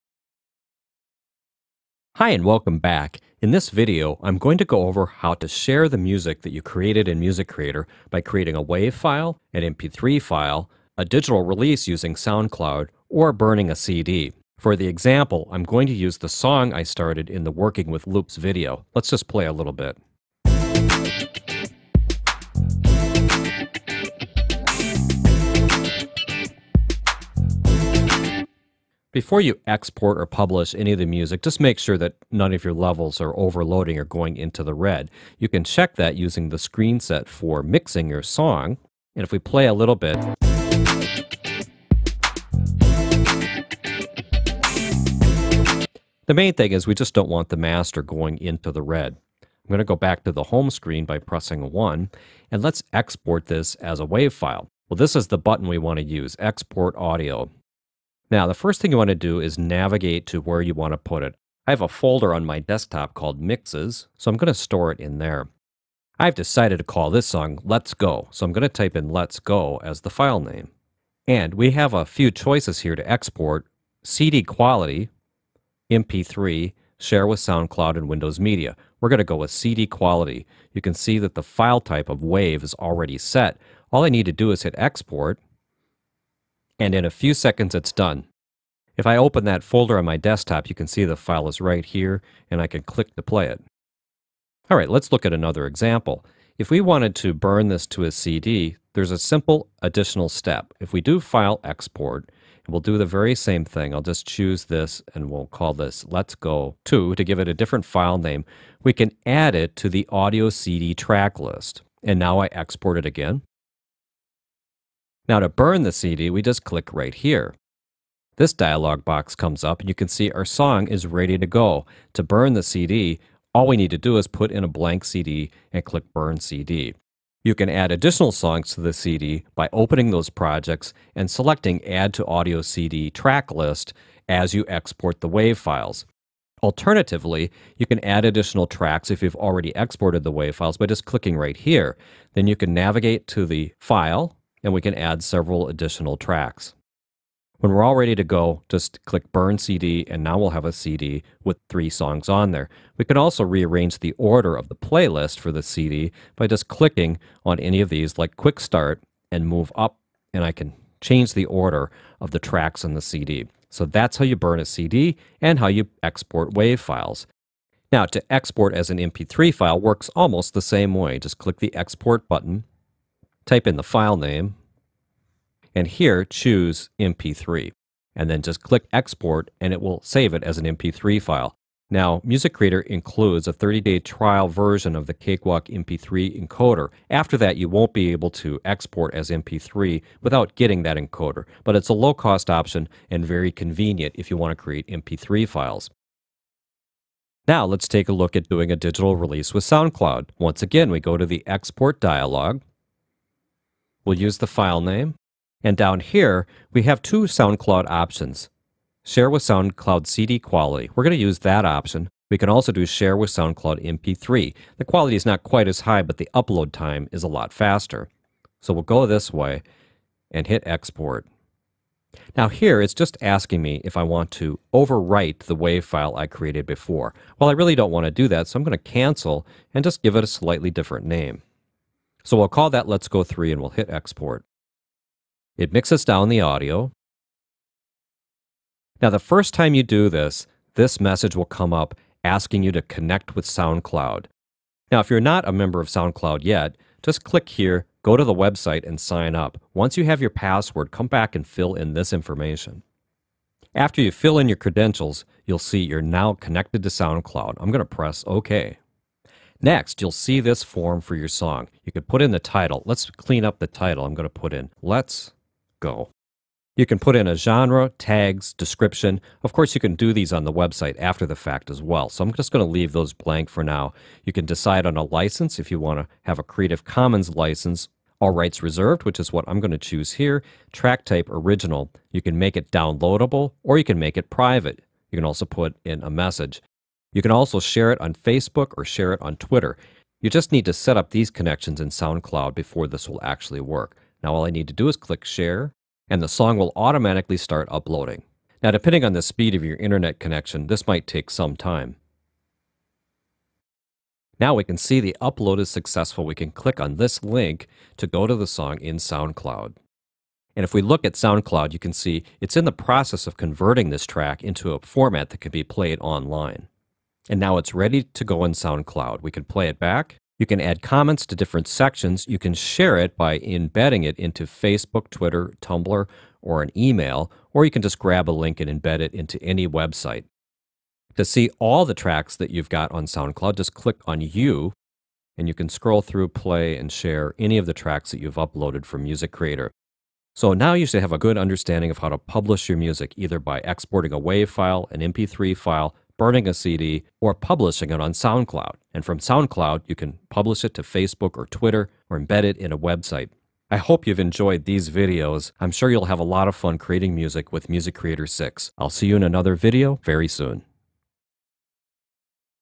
MC6 Part 6: Exporting
Learn how to export and share a song from Music Creator. Includes instructions on how to use the Export Dialog as well as a comprehensive overview of the new SoundCloud feature (account sign up, adding tags, pushing to Facebook and Twitter and more).